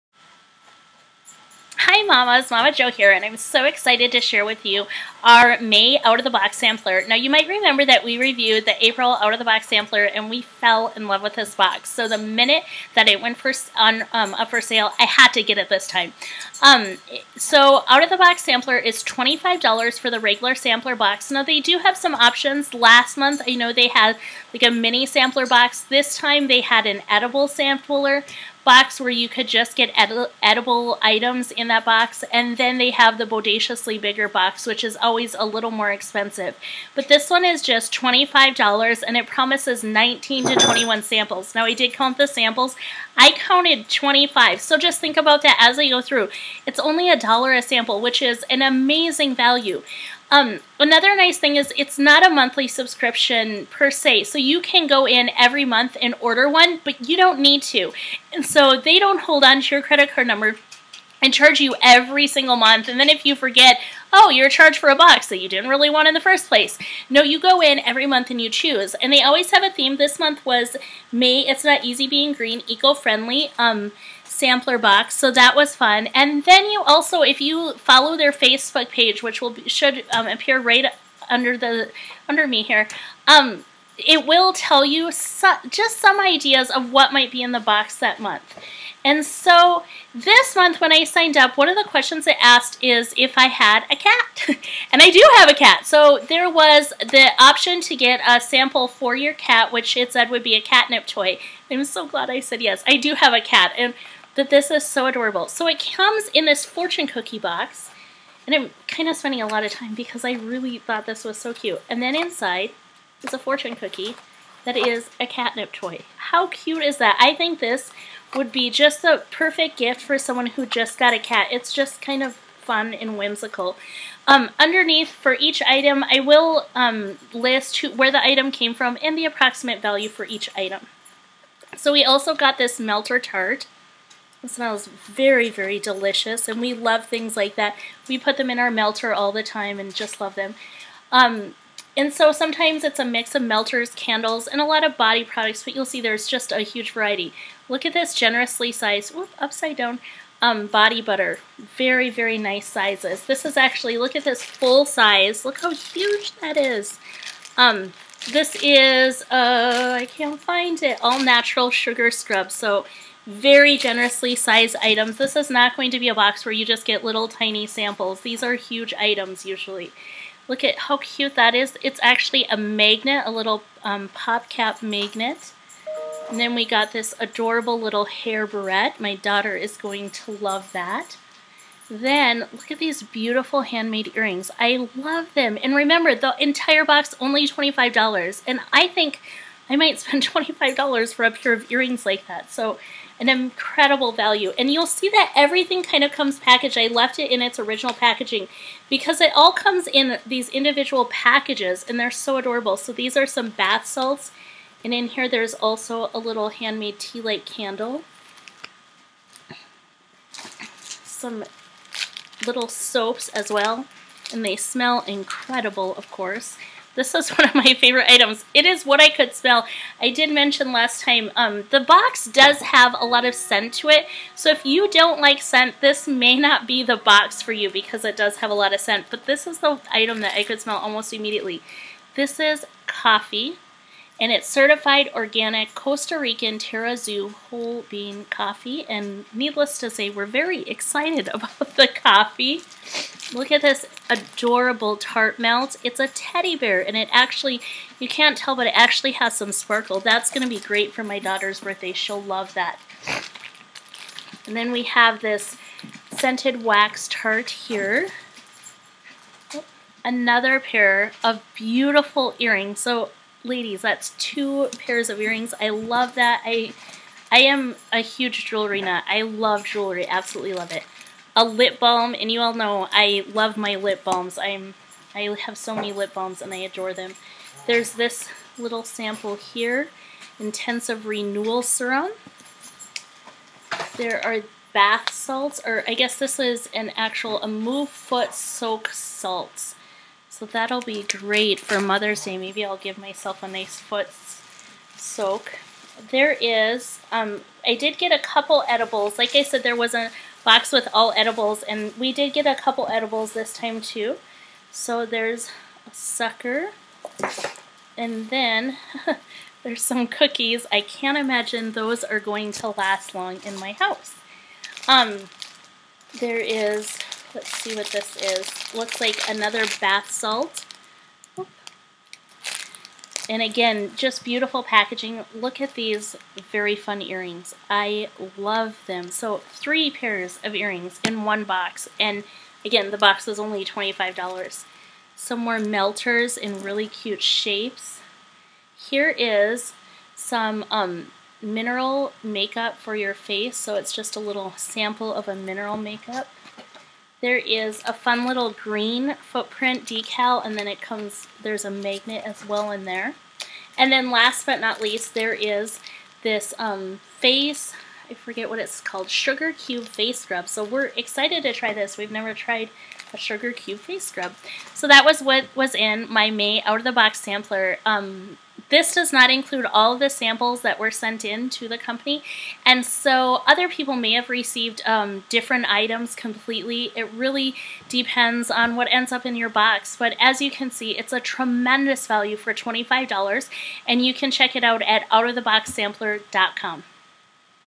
Out of the Box Sampler May 2013
An unboxing of the Out Of the Box Sampler Box for May 2013